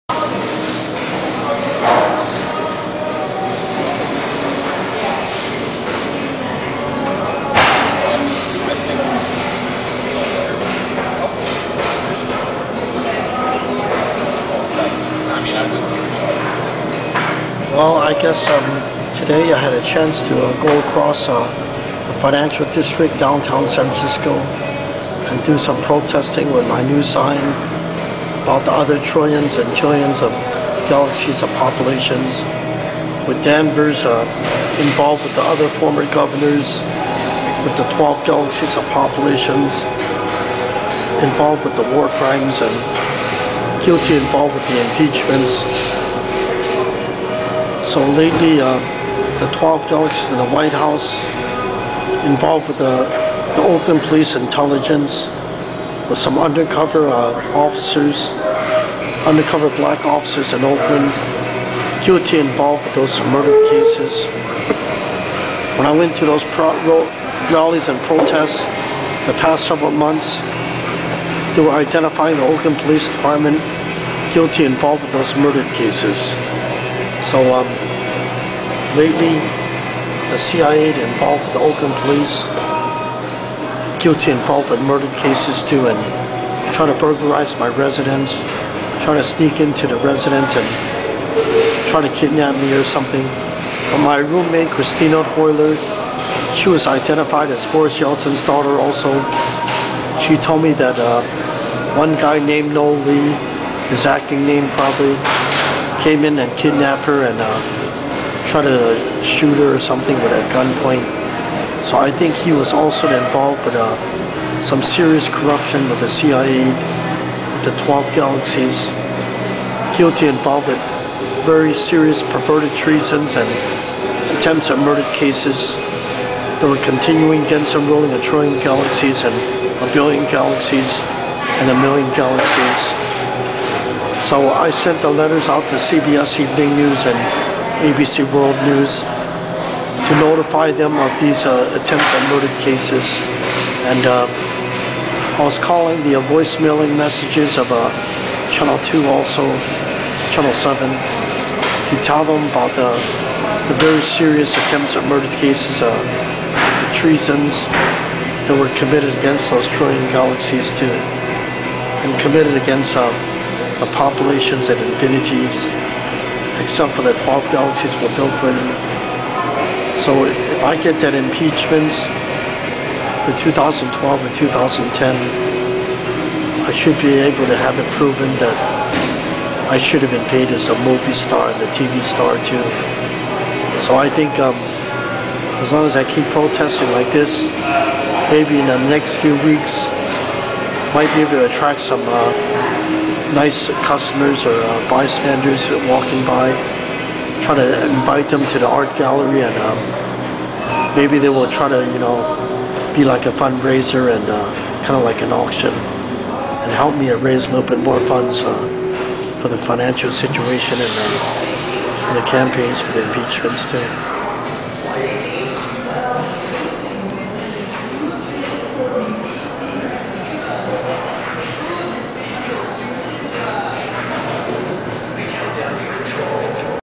ESCAPE from New York. Frank Chu, Pizza, 12 Galaxies in San Francisco
Fran Chu ESCAPE from New York.  Frank Chu, Pizza, 12 Galaxies in San Francisco.

Frank Chu (born 24 March 1960) is one of San Francisco's best-known eccentrics. His street protests against US Presidents, corporations, and "12 Galaxies" have been held in San Francisco and nearby locales since 1995.

Chu lives in Oakland, California, commuting daily to San Francisco,  to find the largest audience of passers-by and television news crews with whom to share his thoughts. He supports himself through a combination of state aid, sign sponsorships, and small donations from his supporters.

Frank Chu holds Bill Clinton responsible for directing the CIA to withhold payment to him for being the star of something called "The Richest Family" during the presidency of George H.W. Bush. His protests frequently called for the impeachment of Clinton even after Clinton was no longer in office. Bill Clinton, George W. Bush and current President Barack Obama are frequently accused by Chu of cooperating with the 12 Galaxies to commit crimes and treasons. Chu is strongly interested in television reporters and newscasters, who will bring him the publicity he requires to inform the world of the injustices committed against him. He hopes that this wave of publicity will cause a public outcry and lead to the impeachment of various US presidents and the awarding of "$20 billions" in compensation for the damages he and his family have suffered.